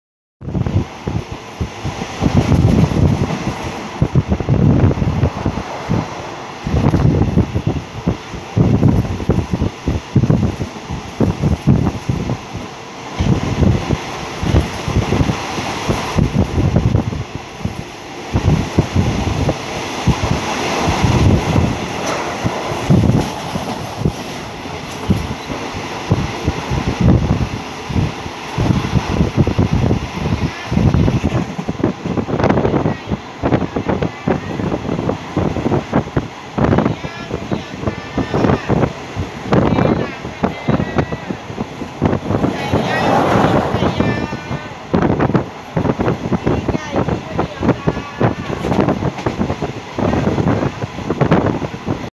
146 ผ่านสถานีหนองน้ำขุ่น
รถเร็ว 146 อุบลราชธานี-กรุงเทพ ผ่านสถานีหนองน้ำขุ่น  เมื่อวันที่ 17 มิถุนายน 2560